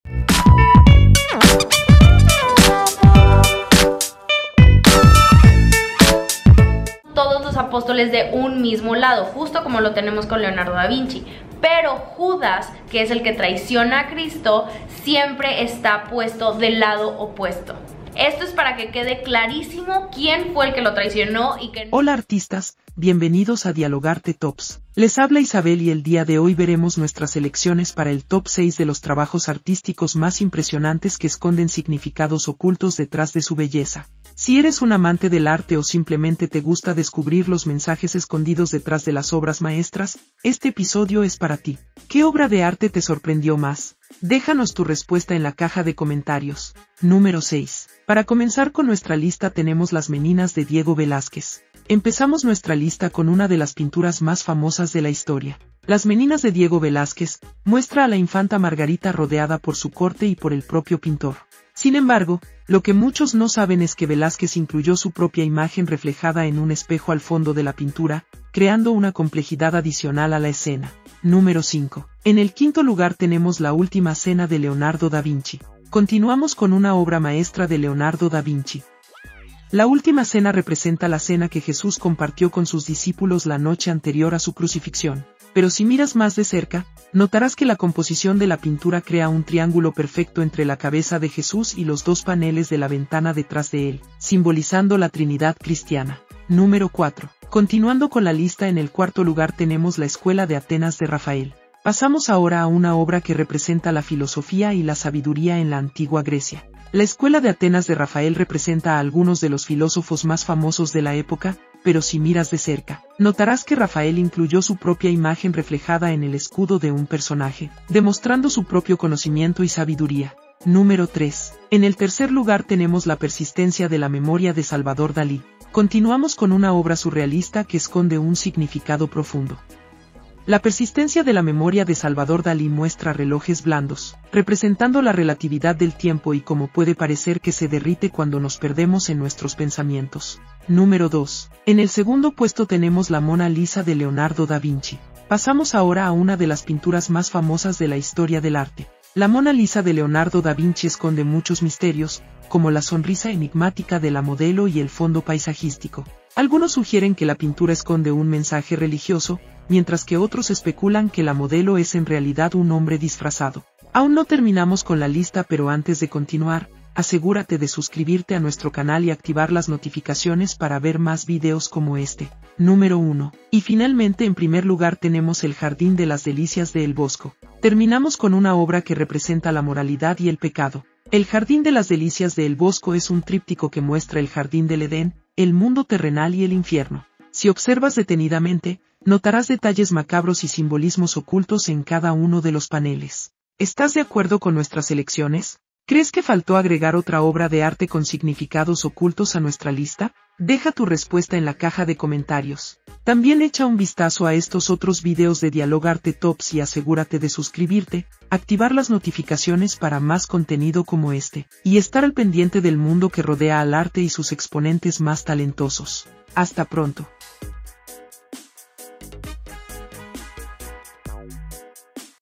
¡Top 6 Obras de Arte con Significados Ocultos!
Para esta lista, veremos las obras de arte que ocultan significados sorprendentes.

¡Sugiere tus ideas en la caja de comentarios!